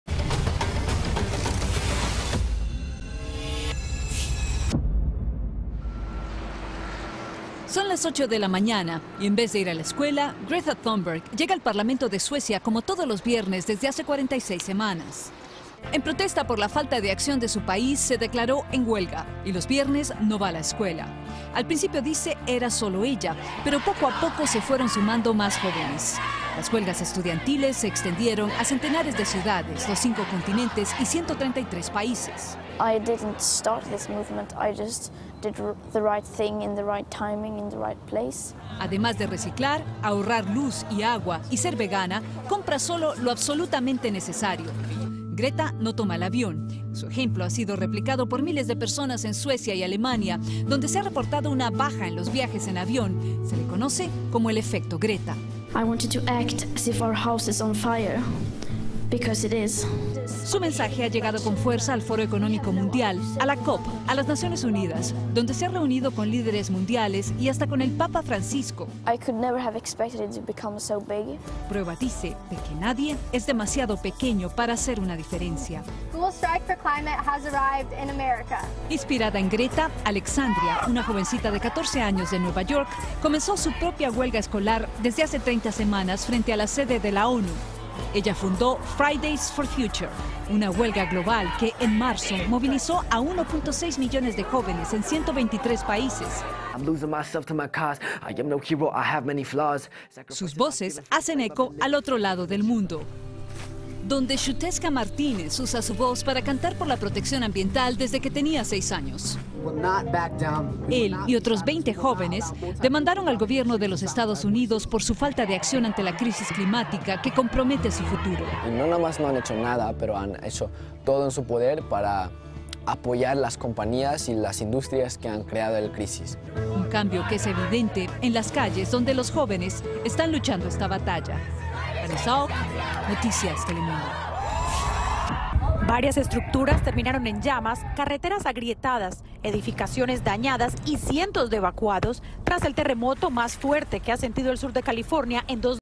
Planeta Tierra: cuando los más jóvenes alzan la voz | Noticias Telemundo
Vídeo oficial de Noticias Telemundo. Millones de niños y adolescentes están exigiendo acciones concretas para proteger a la Tierra; Greta Thunberg es un ejemplo, pero hay muchos más.